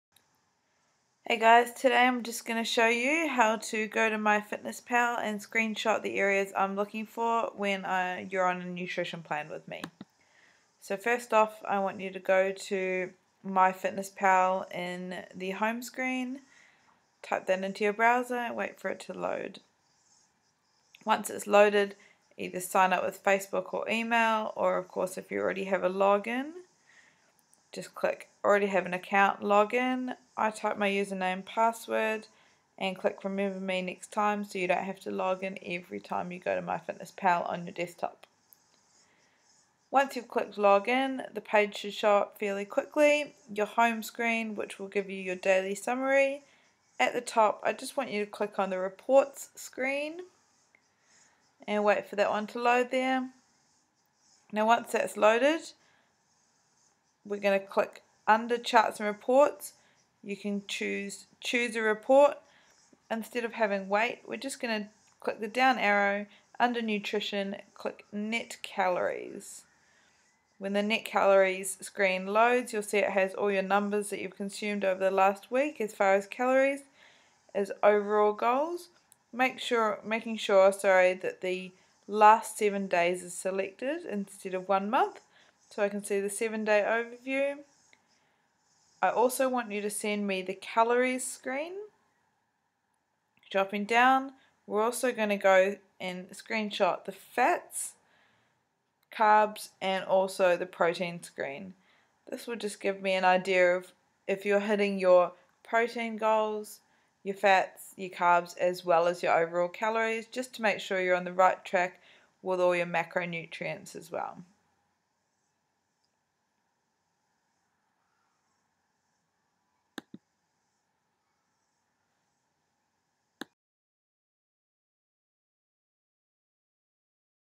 MFP tutorial - how to screenshot the nutrition screen from your desktop
a step by step on how to log onto your my fitness pal and find the nutritional data to send to me for online coaching